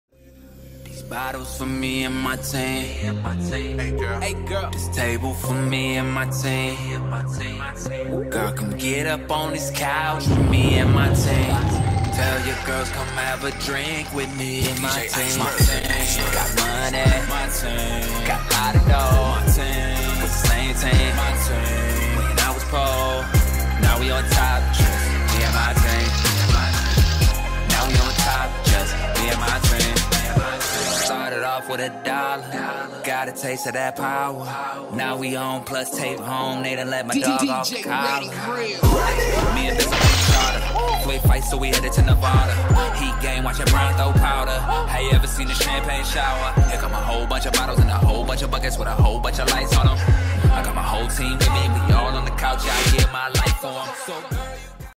BLACKAMERICAAFFAIR
📢📻📱📺💻📡📣📞P.S.A
SATURDAY FEBURARY 21, 2015!!! Kris Thagoodboi Massey CMOB ENT & MASSEYBOYZENT Presents
The First Annual Black History Months "Black America Affair" 🎤Hosted by Thagoodboi!